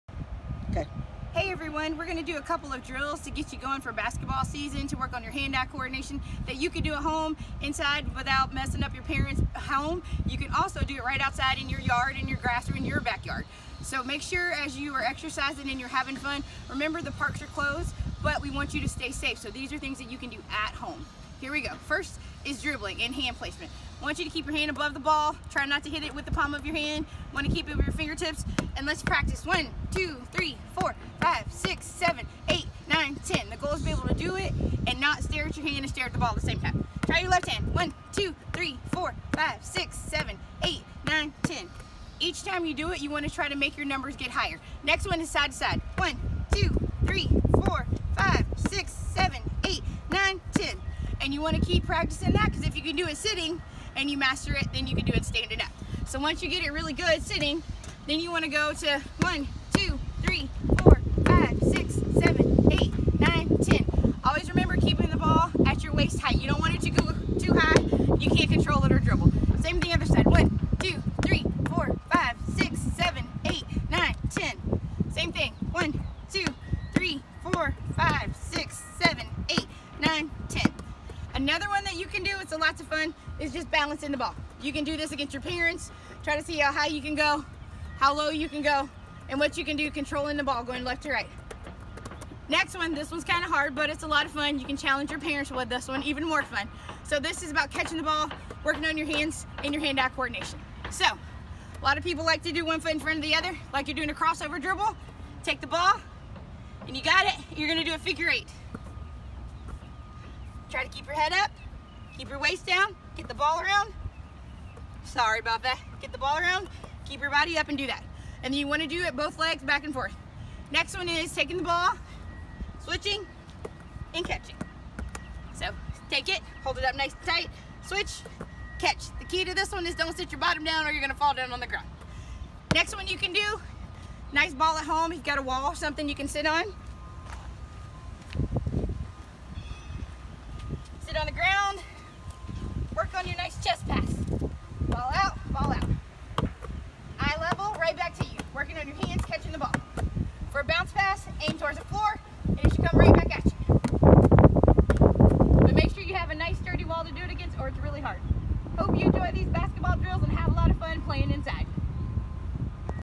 At Home Athletics | Basketball
Mrs. Nancy shows you some drills to do from home!